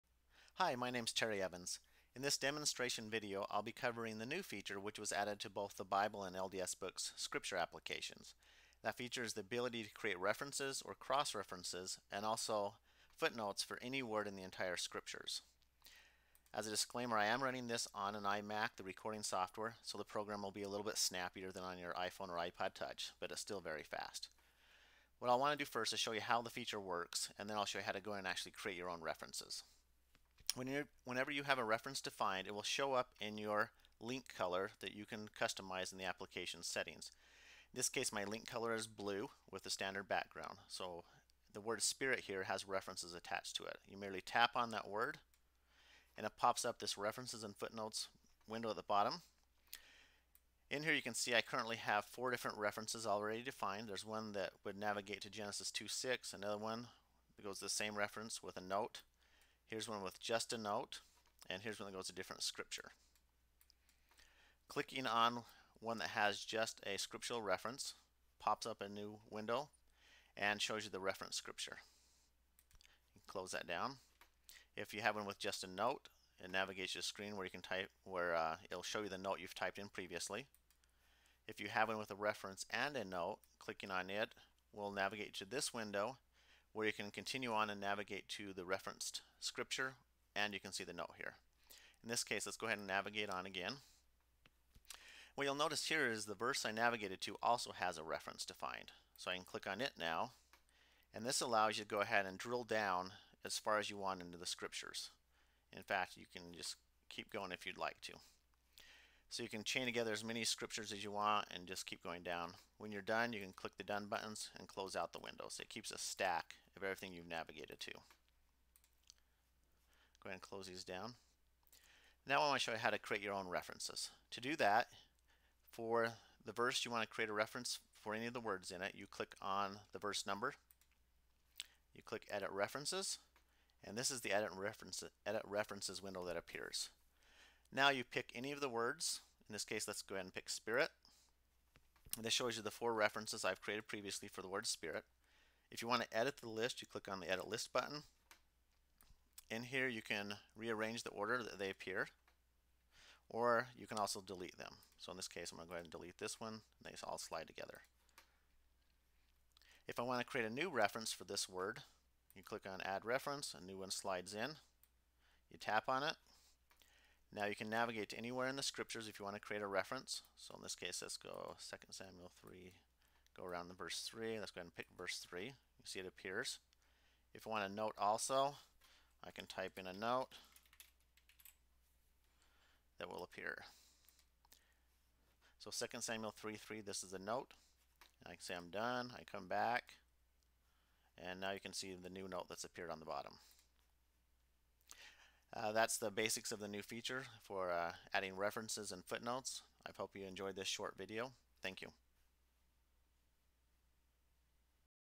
Custom References & Footnotes
Demonstration of the customizable references (cross-reference) and footnotes feature.